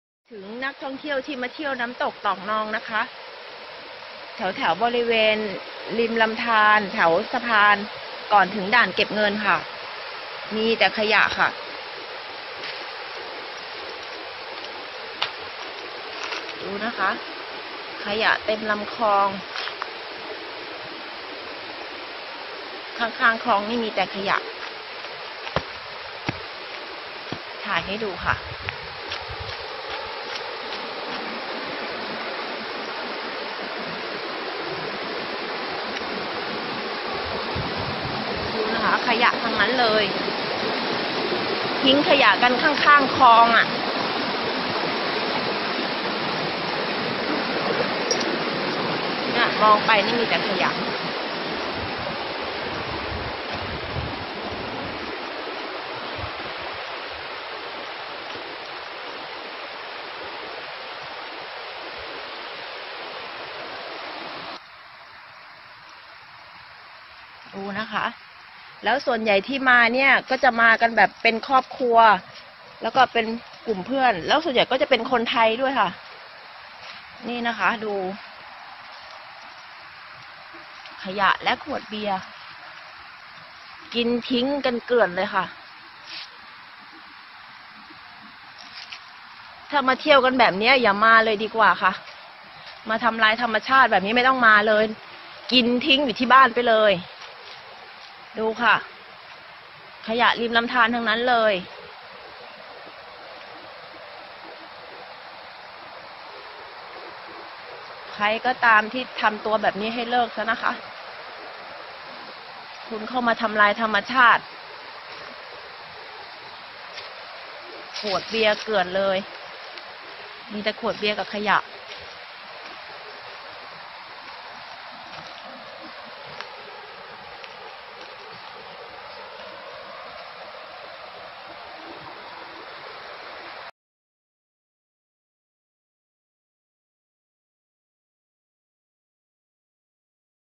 สุดมักง่าย โลกออนไลน์แฉภาพนักท่องเที่ยวทิ้งขยะ-ขวดสุราเกลื่อนน้ำตกตรอกนอง จันทบุรี
29/04/2019 สุดมักง่าย โลกออนไลน์แฉภาพนักท่องเที่ยวทิ้งขยะ-ขวดสุราเกลื่อนน้ำตกตรอกนอง จันทบุรี
 จันทบุรี - นักท่องเที่ยวสุดทน โพสต์ภาพขยะรวมทั้งขวดสุราที่ถูกนักท่องเที่ยวทิ้งเกลื่อนน้ำตกตรอกนอง จ.จันทบุรี หลังพากันเข้ามาพักผ่อนและเล่นน้ำช่วงวันหยุดที่ผ่านมา